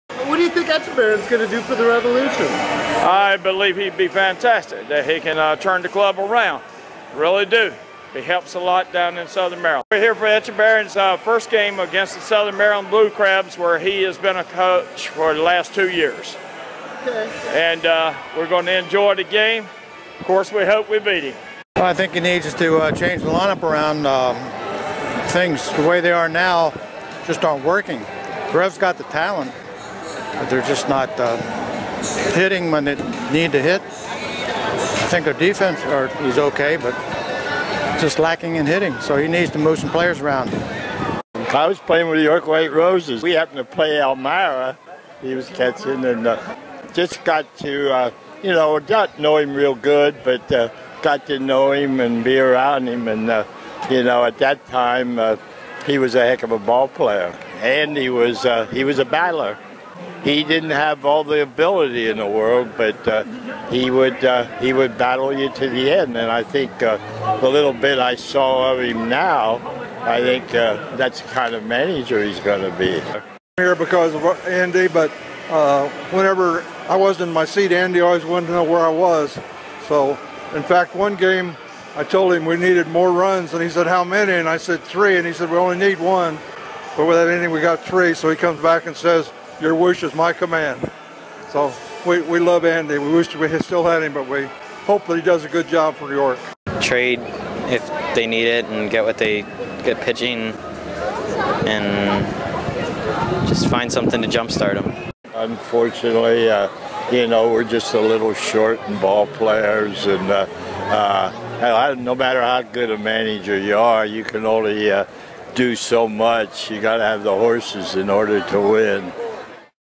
Andy Etchebarren: Fans give their opinion
"What do you think Andy Etchebarren will do for the York Revolution?"